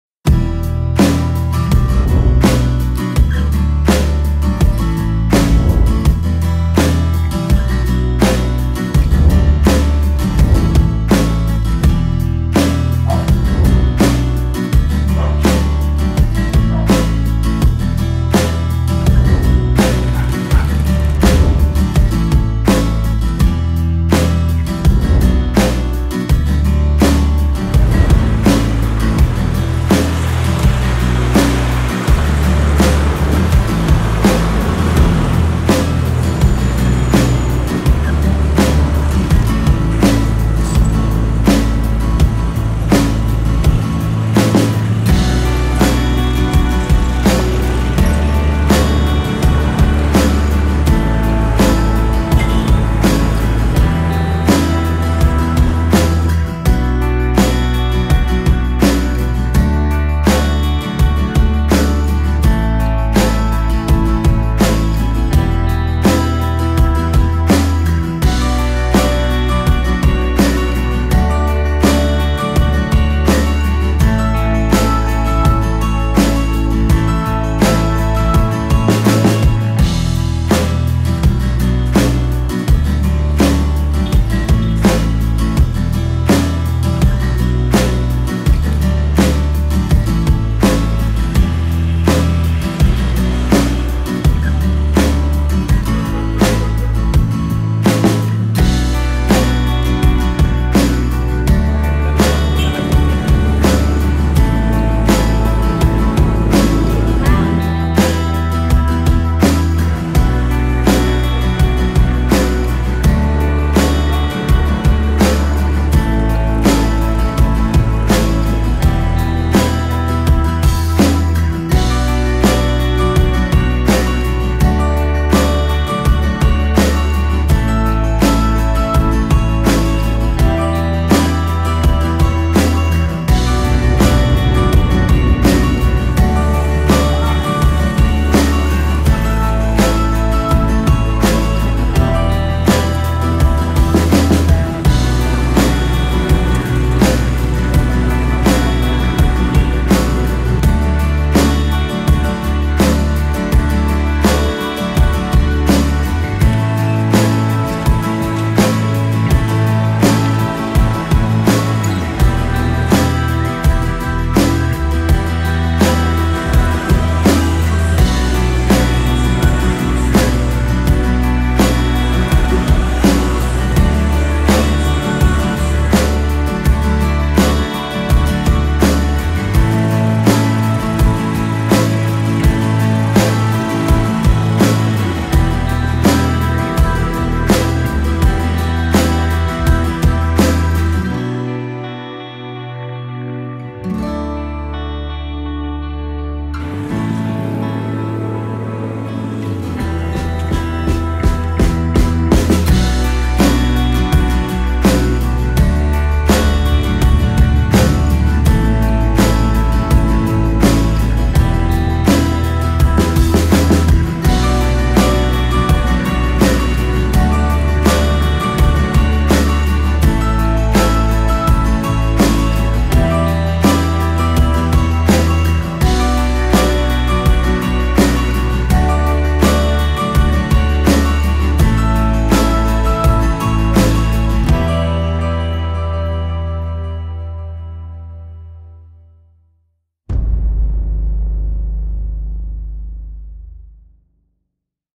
Wakabaut in 8mm Film.
Small short of Closing Sabbath Walk to the Pampanga Capitol in the Philippines.